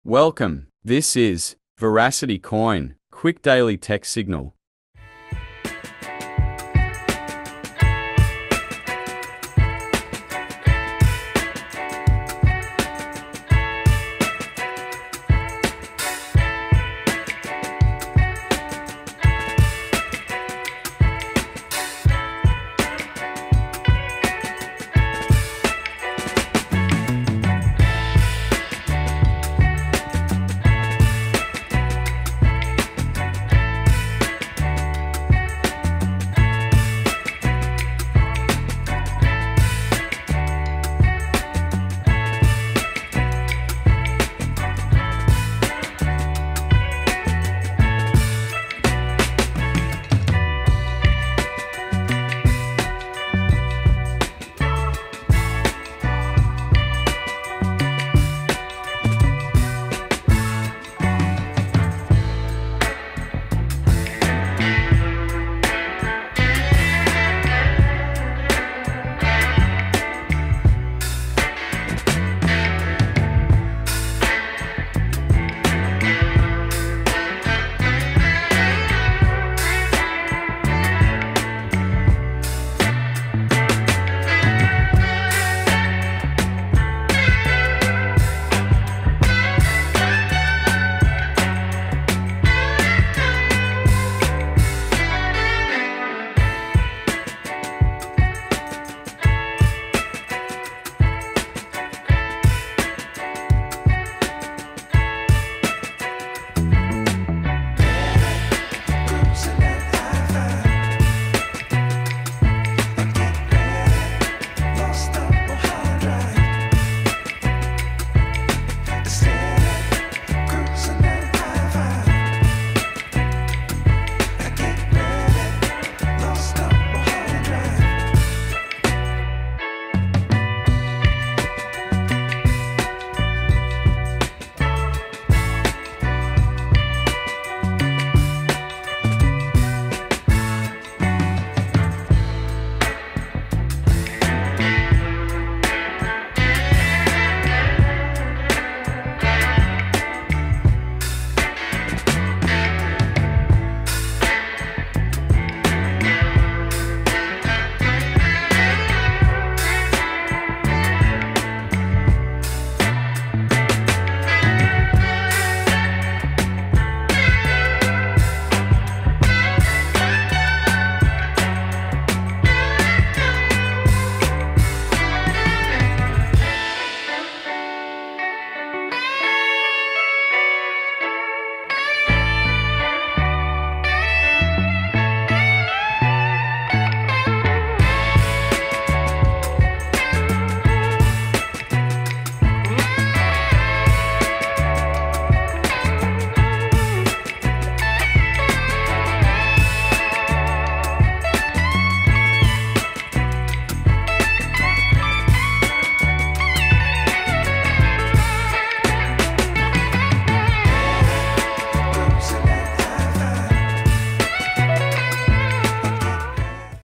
UPDATED || Verasity (VRA) Coin Price News Today - Technical Analysis Price Prediction
In this video, I talk about Verasity coin, VRA coin.